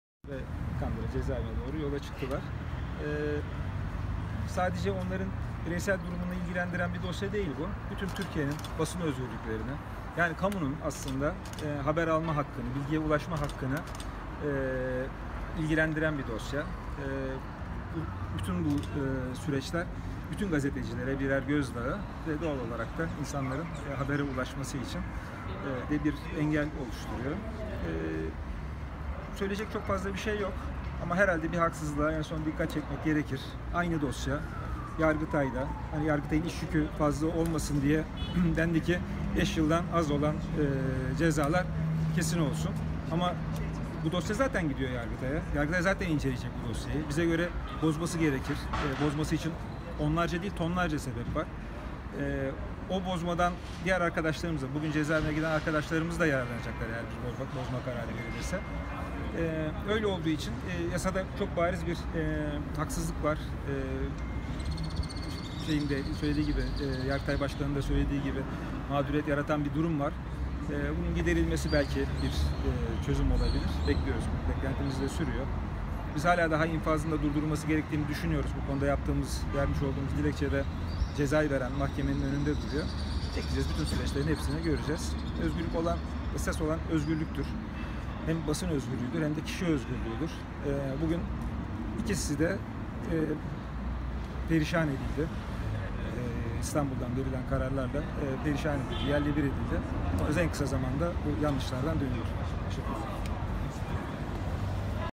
Cumhuriyet çalışanları yeniden cezaevine girdi
Cumhuriyet gazetesinin eski yazar, yönetici ve çalışanlarının yargılandığı davada, 5 yılın altında ceza alanları kapsayan karar, 27. Ağır Mahkemesi tarafından infaz savcılığına bugün yollandı.